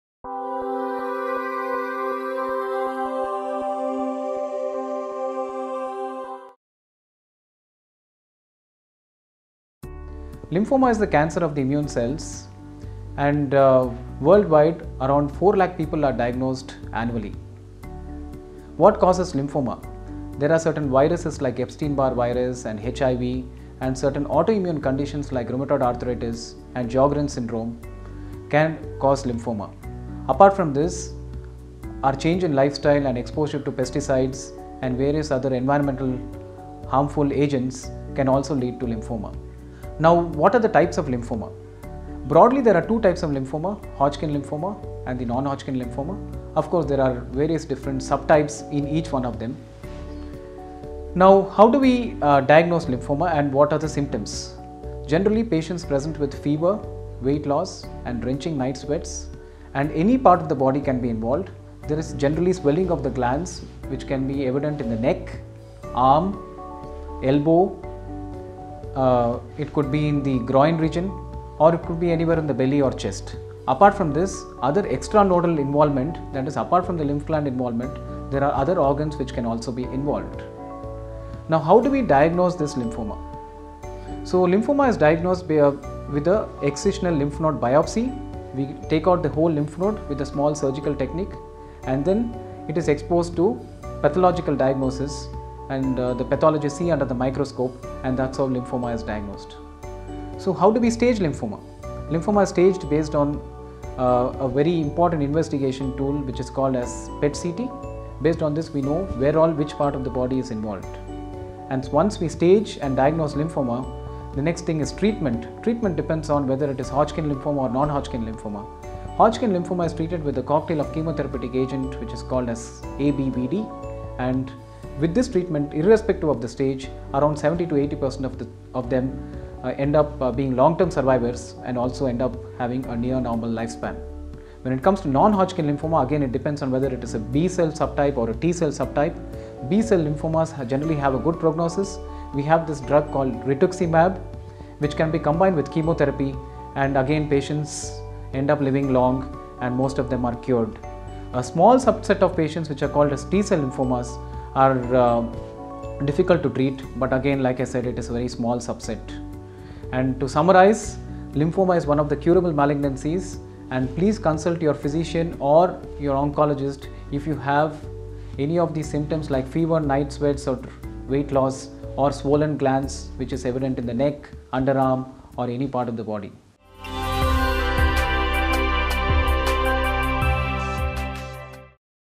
How bad is Lymphoma Cancer?
In this video Dr. Vishwanath S, explains the causes and treatment of Lymphoma cancer.

Lymphoma is cancer that begins in infection-fighting cells of the immune system, called lymphocytes. These cells are in the lymph nodes, spleen, thymus, bone marrow, and other parts of the body. When you have lymphoma, lymphocytes change and grow out of control.

World-wide around 4lacs. Of people are annually diagnosed with Lymphoma cancers. Main causes of lymphoma are some of the virus like Epstein Barr virus, HIV and certain conditions like rheumatoid arthritis can cause lymphoma apart from this change in lifestyle and exposure to pesticides and other various environmental harmful agents can cause lymphoma.

This video also covers the types of Lymphoma, which are mainly classified into two types - Hodgkin Lymphoma & Non-Hodgkin Lymphoma, there are further sub-types.

Dr. Vishwanath S, Consultant, Medical Oncology (Apollo Hospitals, Bannerghatta Road) has experience of Clinical Fellow, Department of Lymphoma/Myeloma, Division of Cancer Medicine, The University of Texas MD Anderson Cancer Center, Houston, TX.